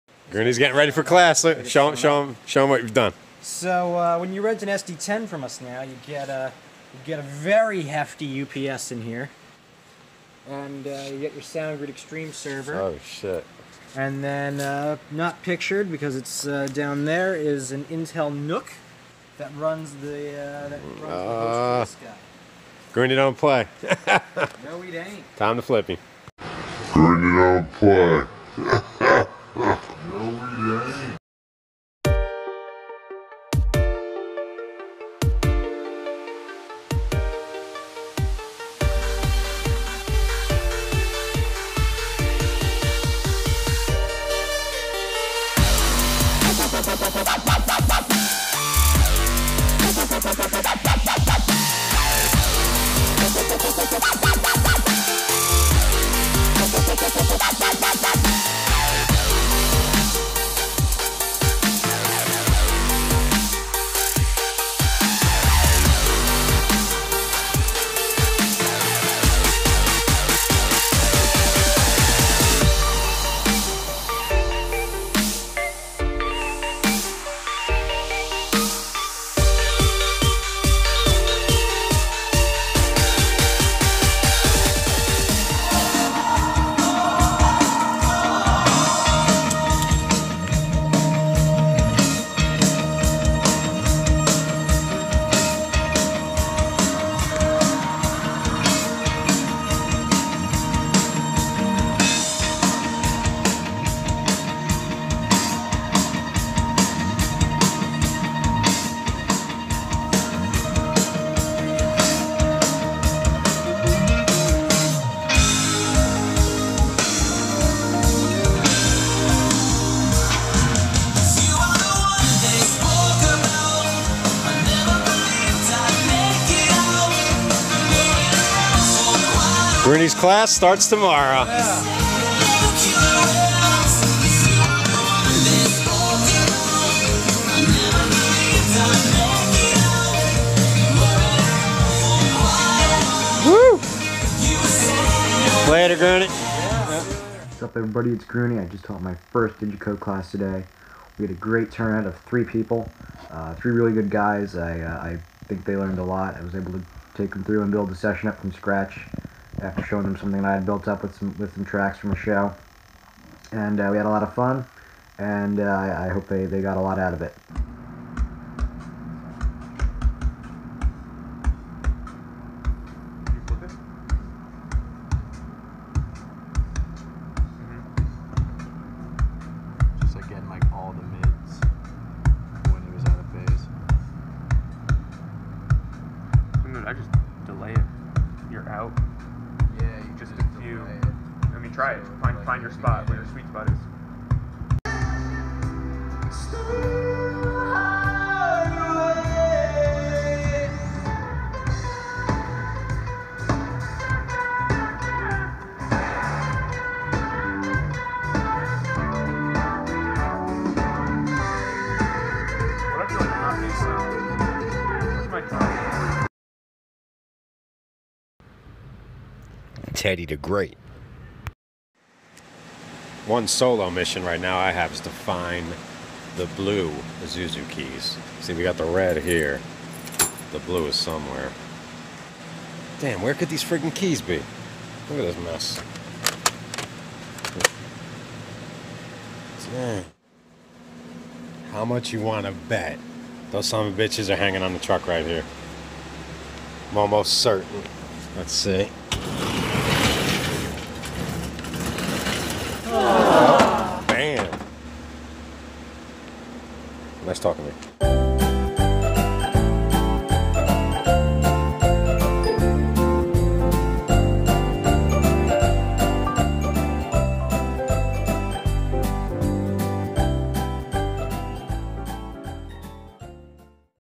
Sound Shop Vlogs ep4 - Professor Grunie's Digico Class Audio Vlog Console Talk Production Company
Weekly Work Vlogs featuring K, Grunie, Marcus, Ted, and many others.

Today we show highlights of Grunie's Digico Day at the shop.